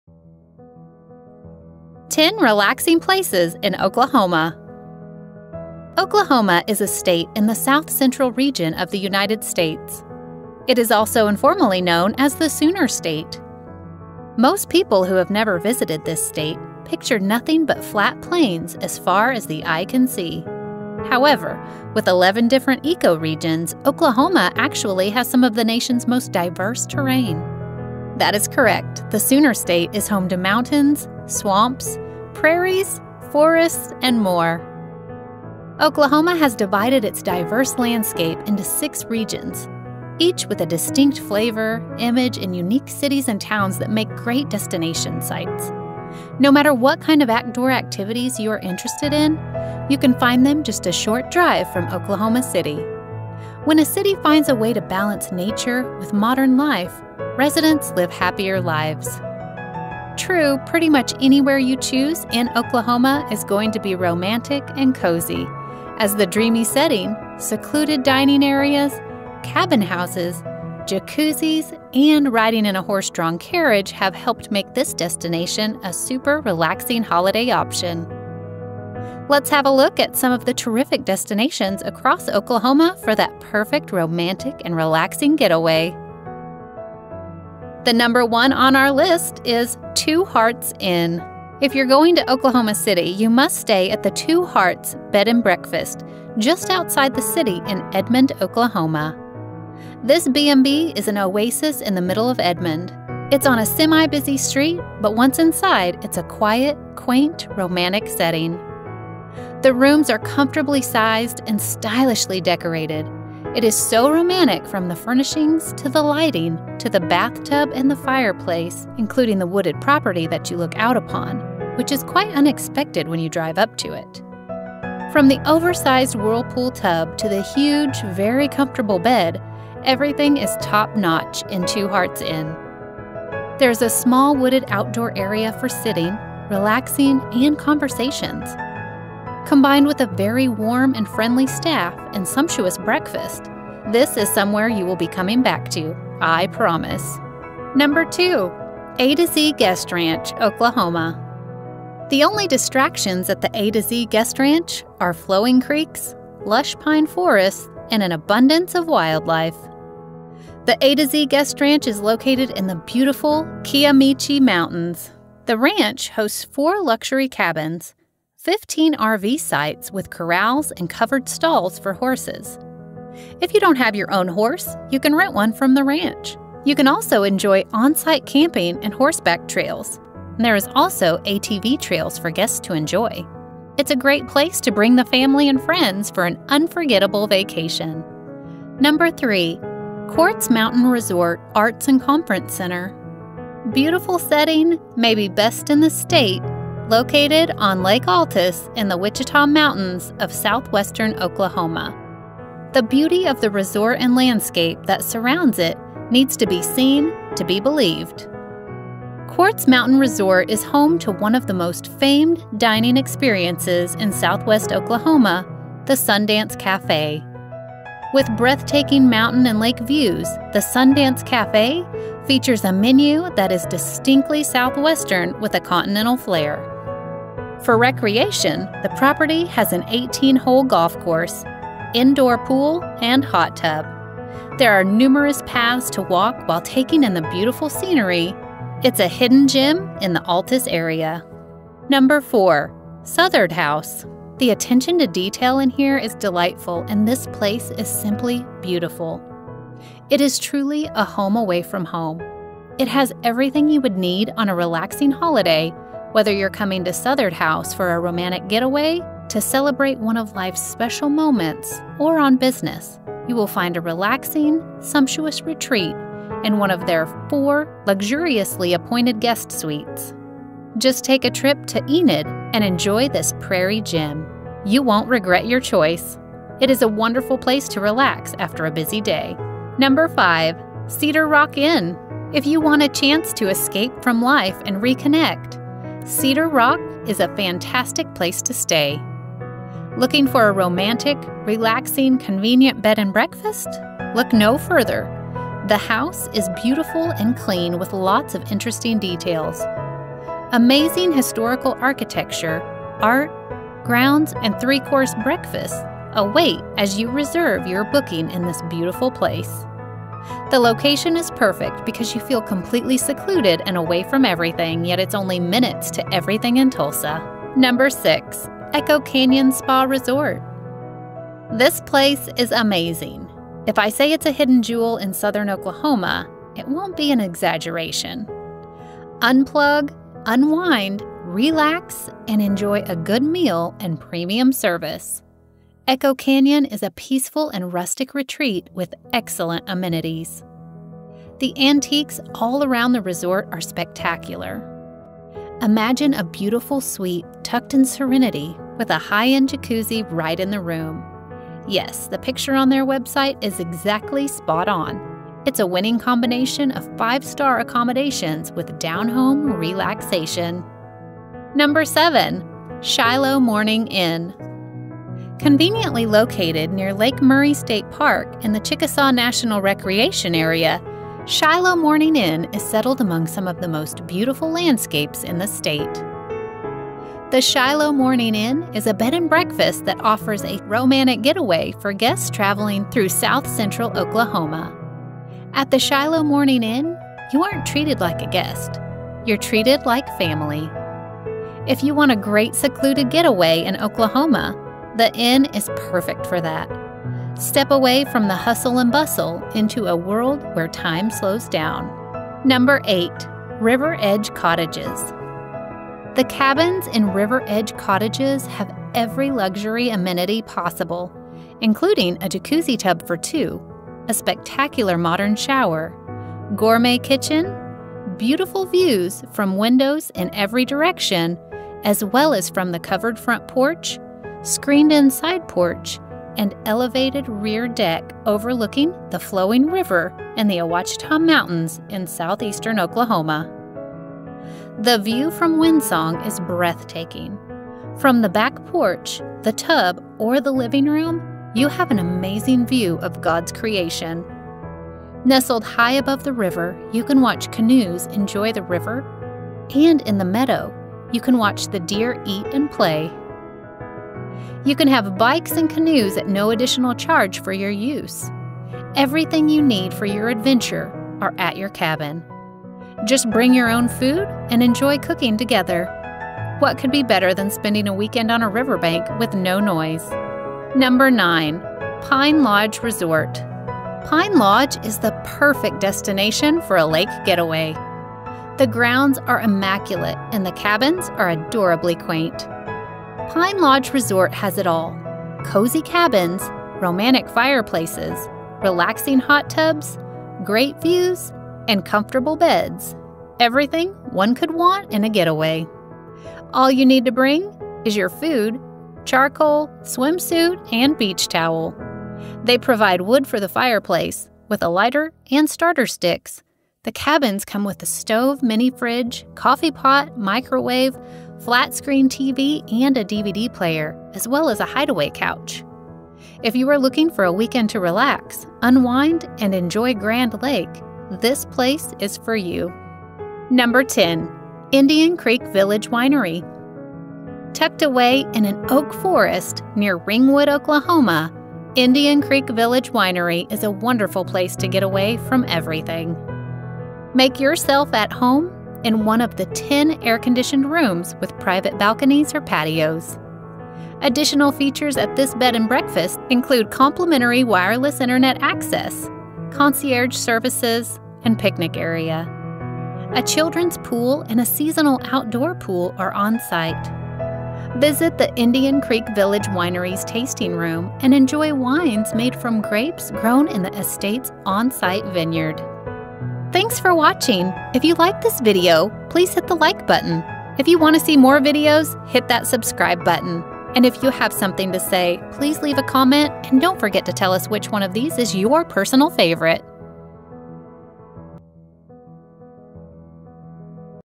👍10 Relaxing Places To Visit In Oklahoma
10 Relaxing Places To Visit In Oklahoma

Get a deal on hotel bookings and flights worldwide with free cancellations on most rooms!

Oklahoma Is A Wonderful State Which Is Located In The South Central Region Of United States Of America. The Capital Of This Sate Is Oklahoma City. Like The Other States Of America, Oklahoma Also Has Various Attractive RV Campground And Resorts Which Provide The Best Facilities And Amenities To All The People Who Come Here.

Relaxing Places In Oklahoma .